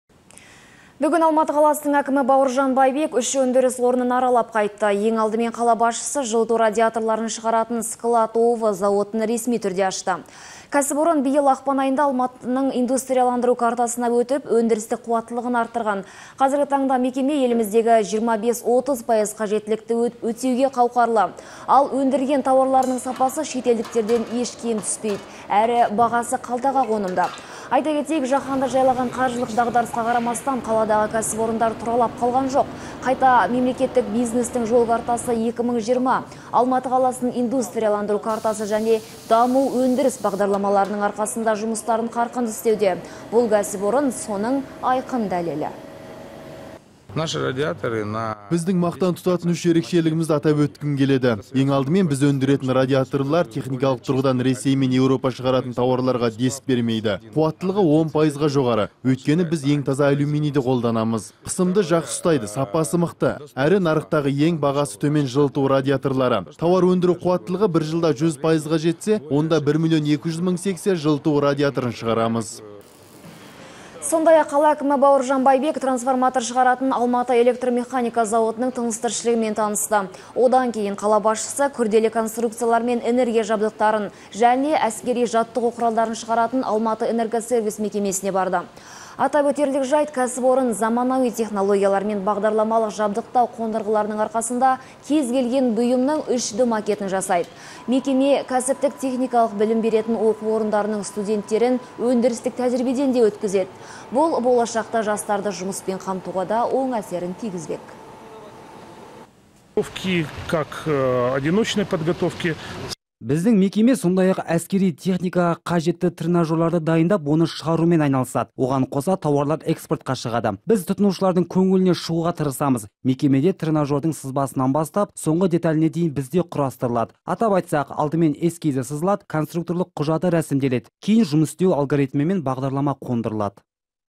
Алматы әкімі жылыту радиаторларын шығаратын зауытты ашты (29.03.16)
Қазіргі таңда мекеме еліміздегі 25-30 пайыз қажеттілікті өтеуге қауқарлы. Ал,  өндірген тауарларының сапасы шетелдіктерден еш кем түспейді.